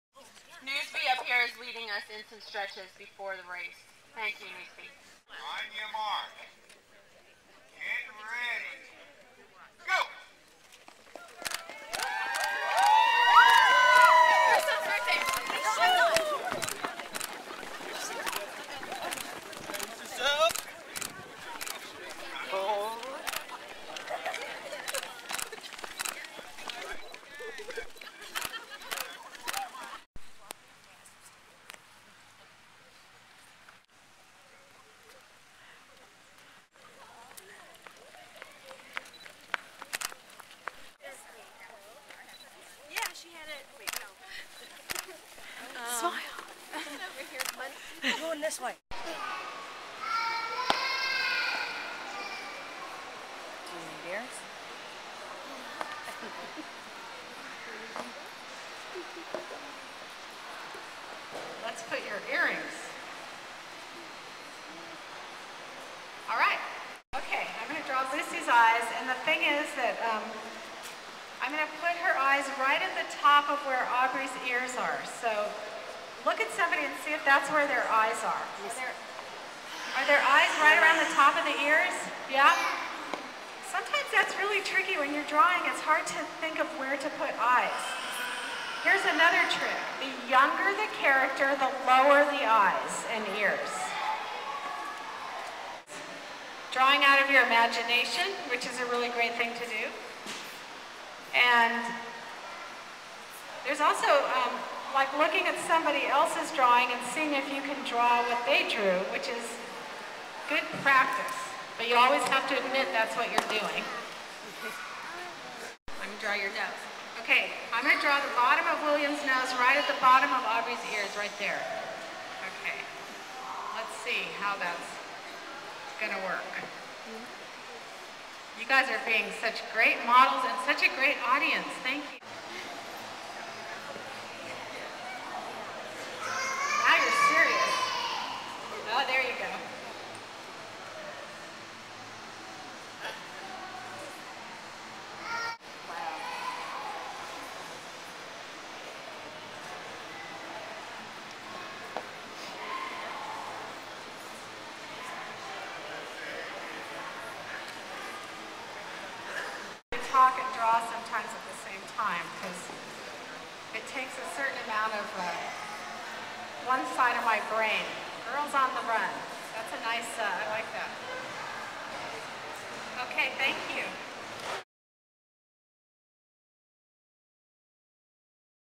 2009 Run to Read
The 2009 Run to Read took place Saturday, Oct. 10, at Lions Lake in Washington. Caldecott Medal Honoree Marla Frazee, a four-time Book Buzz Pick author, attended the race to share her new children's book, "All the World." This marked the fourth year for the Run, which is organized by the Four Rivers Area Family YMCA and The Missourian.